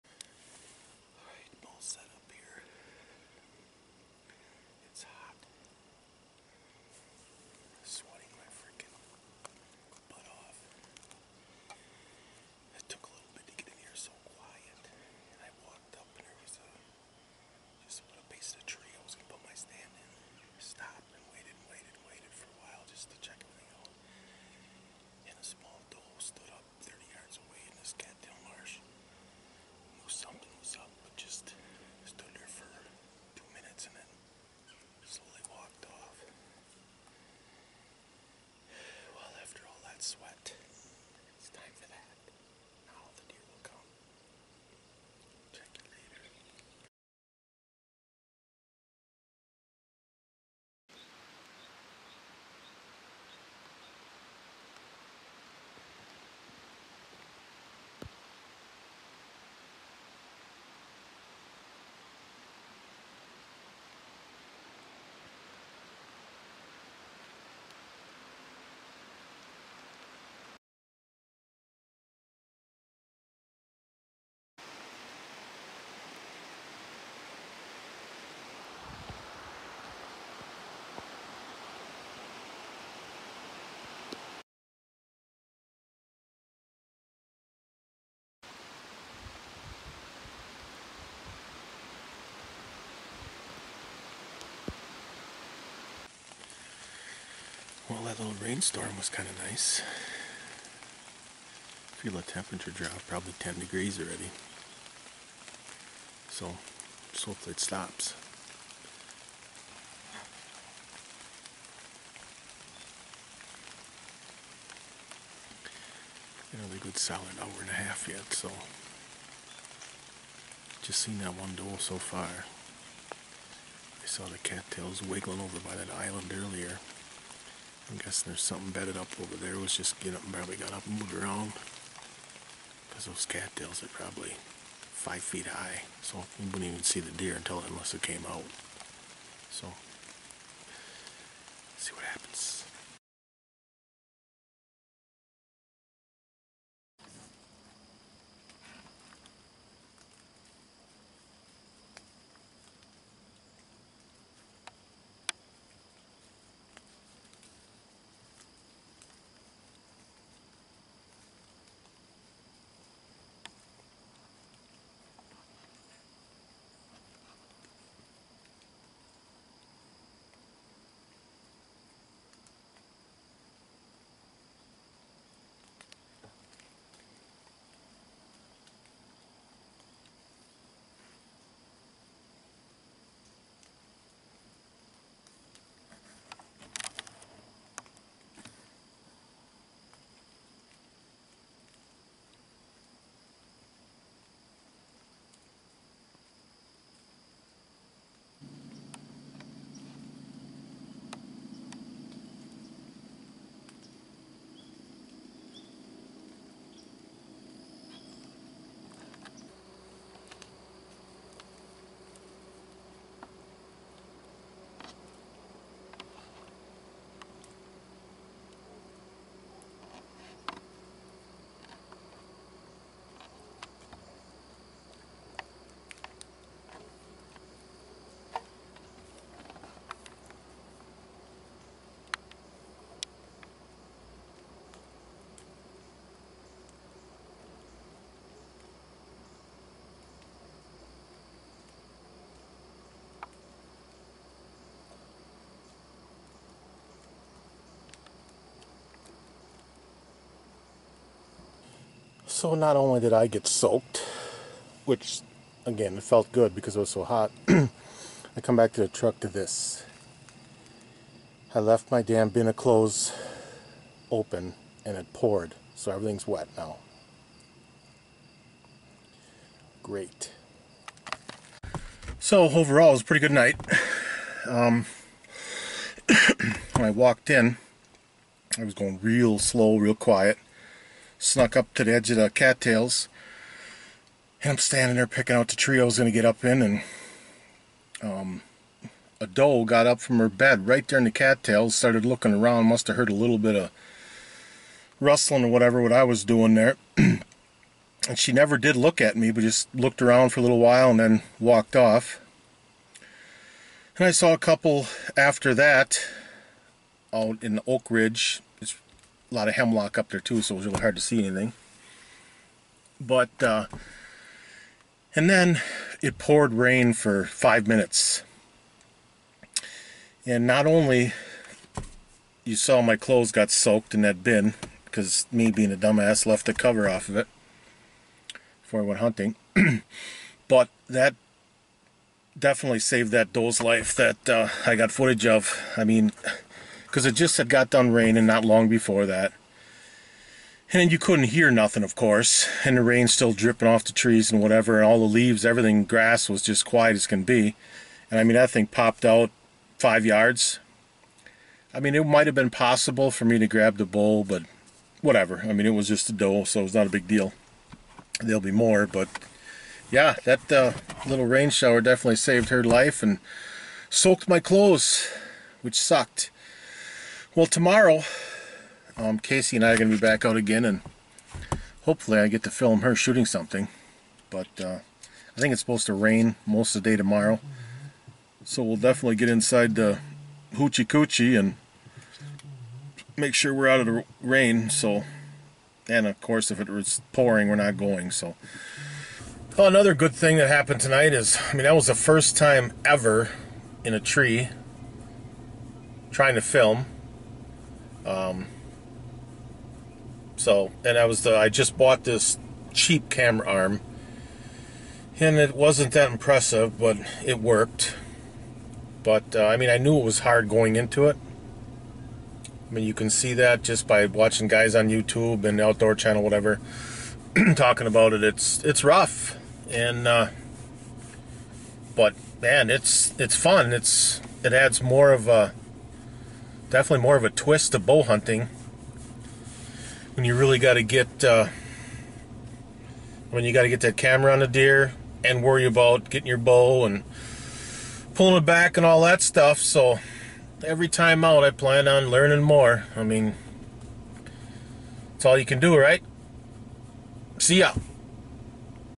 First time self filming.
This was my first time self filming a hunt. Learned a ton and expect to learn more each time out.